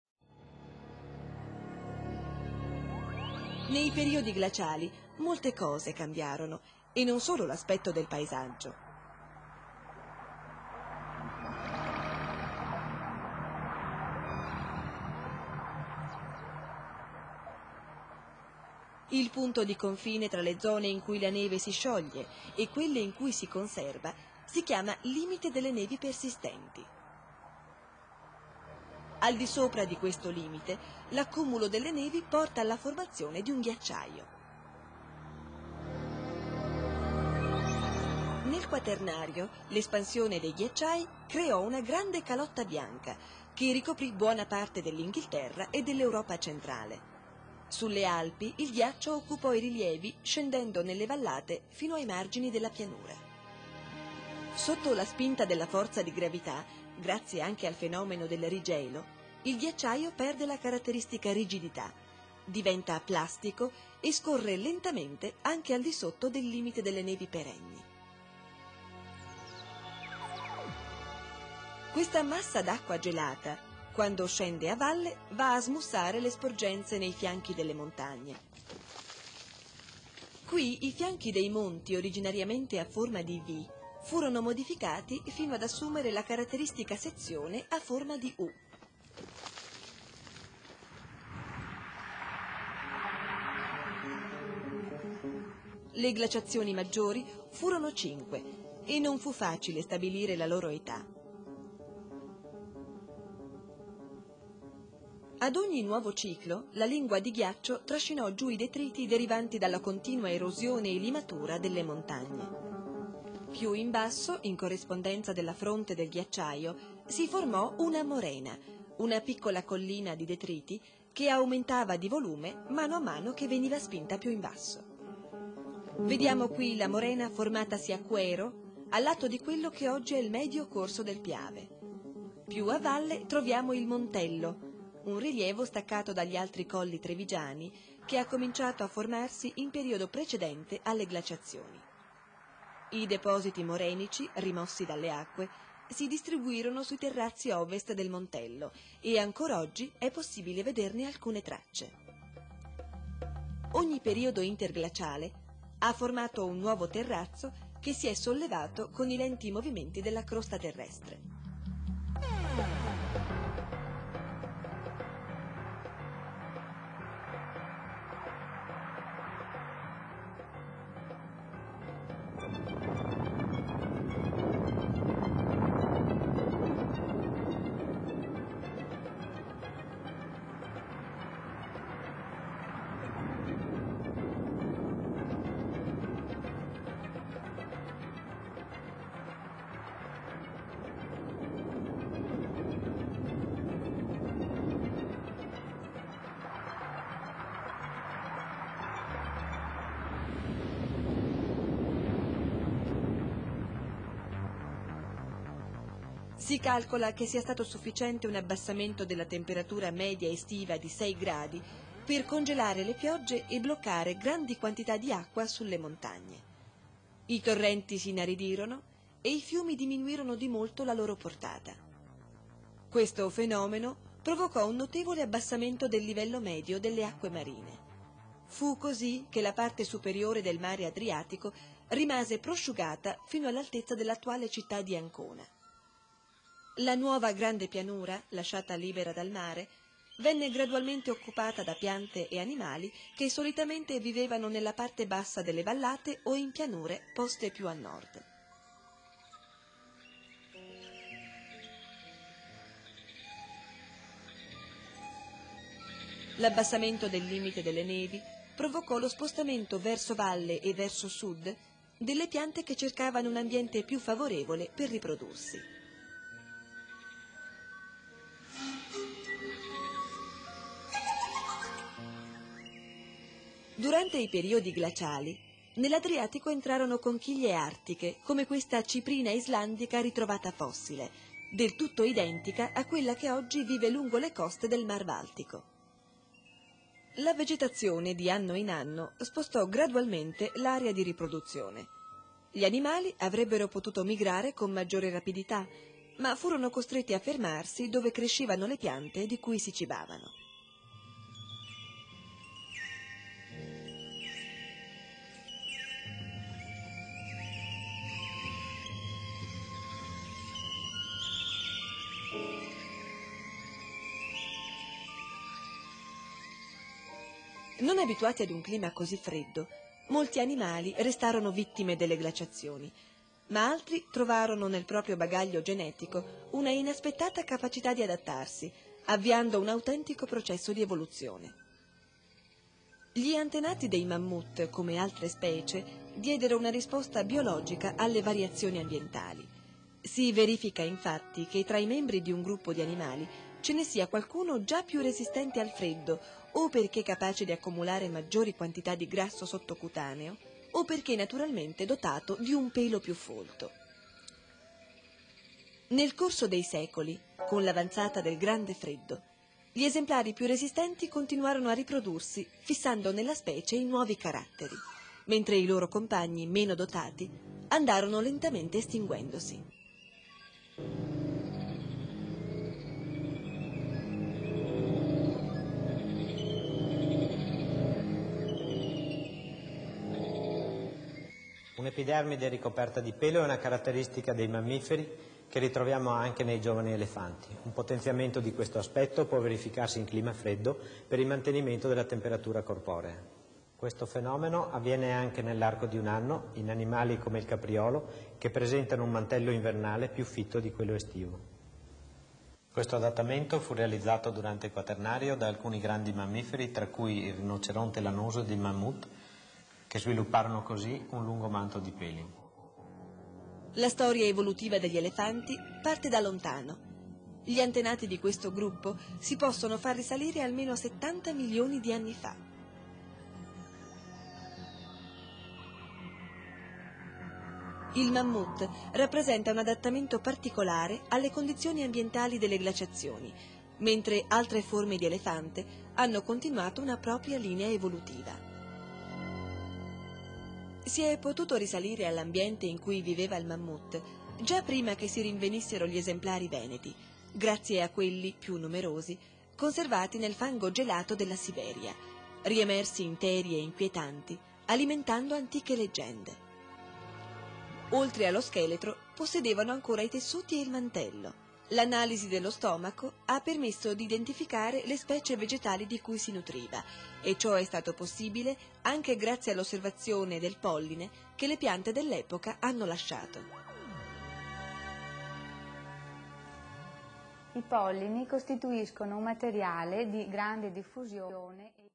Mammut Pordenone 5/6
Il video realizzato in occasione dell'allestimento della mostra sul Mammut a Pordenone.